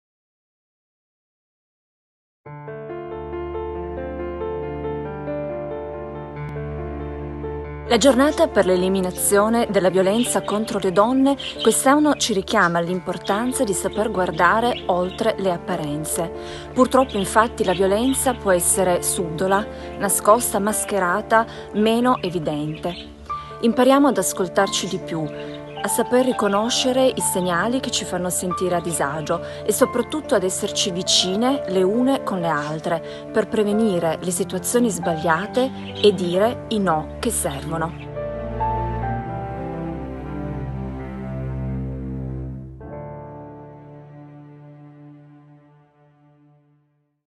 Le consigliere regionali contro la violenza sulle donne (2)
Anche quest'anno in occasione del 25 novembre, Giornata internazionale per l’eliminazione della violenza contro le donne, il Consiglio regionale rinnova la campagna sociale sulla prevenzione e il contrasto della violenza. Ecco il messaggio della consigliera Monica Canalis a tutte le donne.